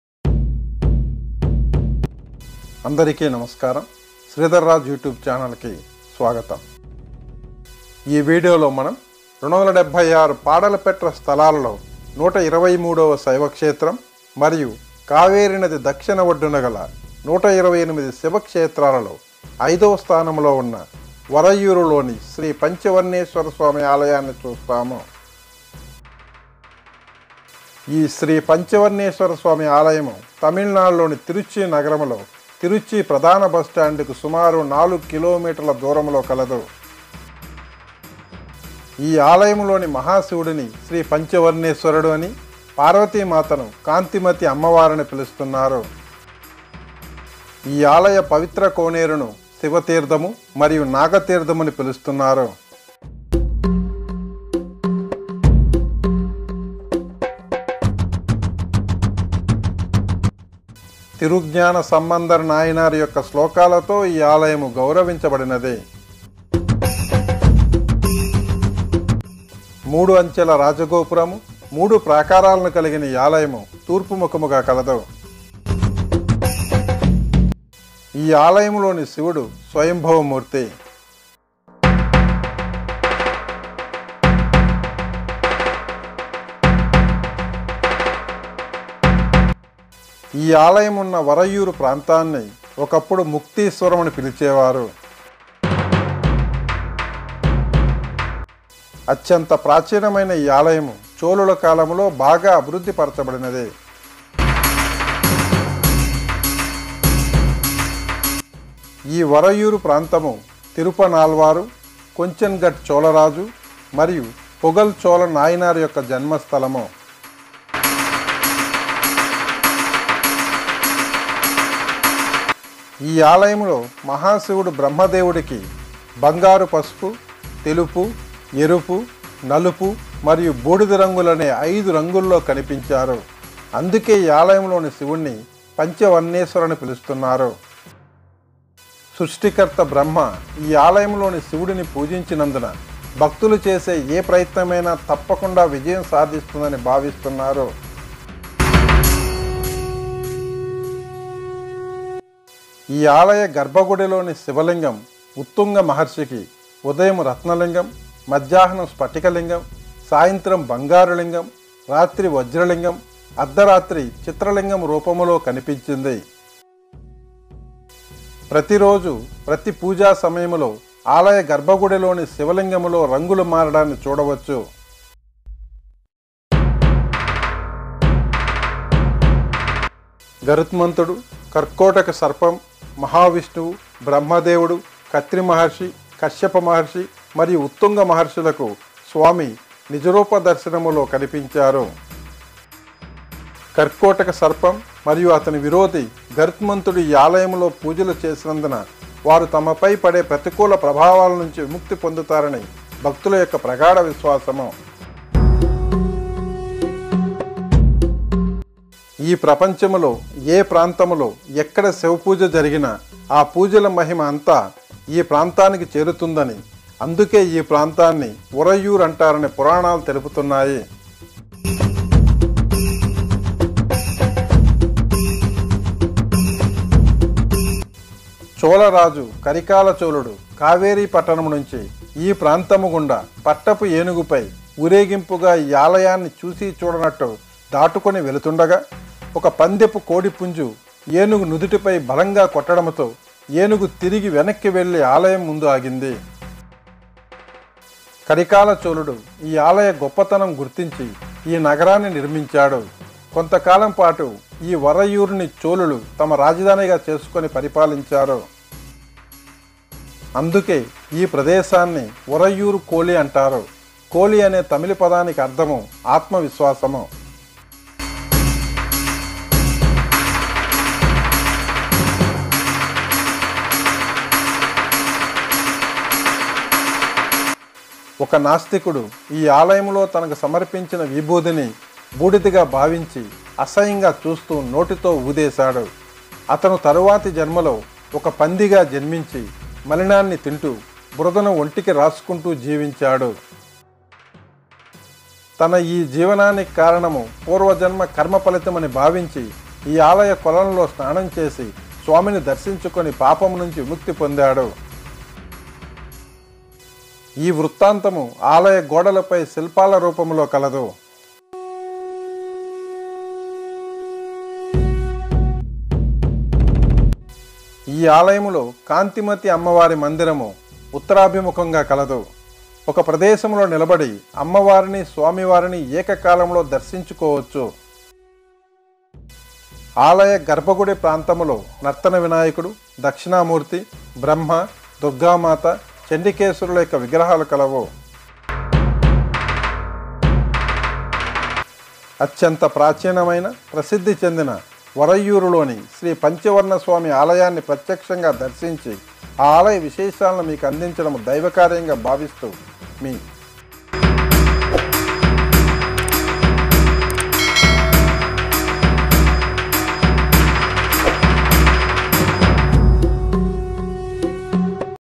123 Siva kshetram/Sri Panchavarneswarar Swamy temple/WORAIYUR/276 Paadal Petra Sthalam/Sreedhar Raju
ఈ వీడియో లో మనం 276 paadal petra sthalam లలో 123 వ శైవక్షేత్రం మరియు కావేరినది దక్షిణ ఒడ్డున గల 128 శివక్షేత్రాలలో 5 వ స్థానంలో ఉన్న వరైయుర్ లోని శ్రీ panchavarneswar స్వామి ఆలయాన్ని చూస్తాము.
ఈ ఆలయము తమిళనాడు లోని trichy నగరంలో trichy central busstand కి సుమారు 4 km దూరంలో కలదు.
ఈ ఆలయము లోని మహాశివుడిని శ్రీ panchavarneswar అని,పార్వతిమాతను కాంతిమాత అమ్మవారు అని పిలుస్తున్నారు.
ఈ ఆలయ పవిత్ర కొనేరుని శివతీర్థం, మరియు నాగతీర్ధం అని పిలుస్తారు.
తిరుగ్యన సంబంధర్ నాయనార్ యొక్క శ్లోకాలతో ఈ ఆలయము గౌరవించబడింది.
ఈ ఆలయము లోని శివుడు స్వయంబువ మూర్తి.